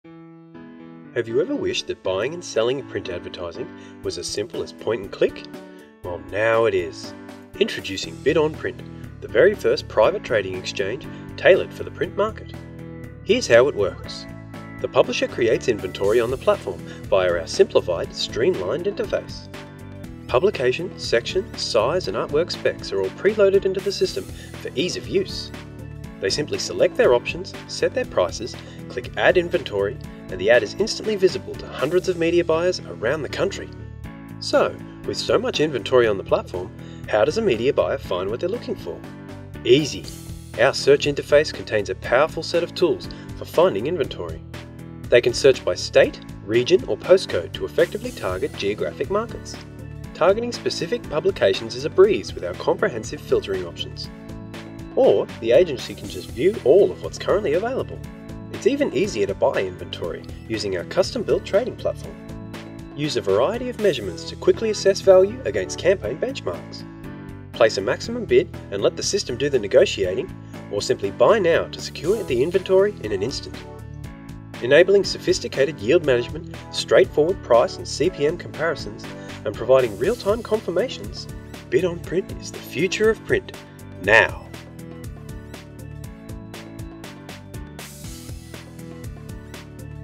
Introducing Bid on Print
Bid on Print is a private exchange focused on the purchase of advertising in print newspapers. Bid on Print provides direct access to publication inventory, circulation and readership comparisons and a simplified purchase process.